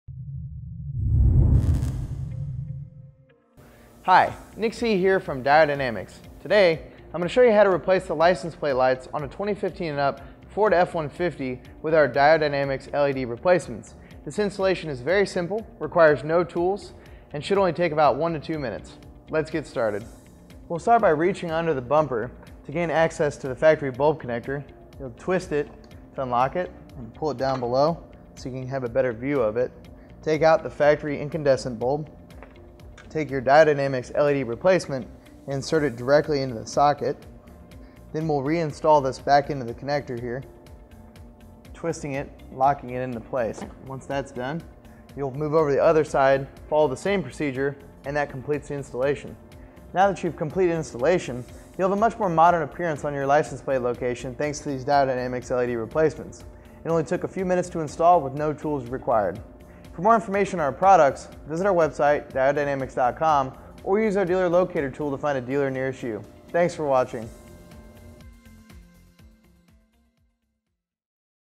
How to Install 2015+ Ford F-150 License Plate Lights
This video demonstrates the simple installation of license plate lights on the 2015+ Ford F-150.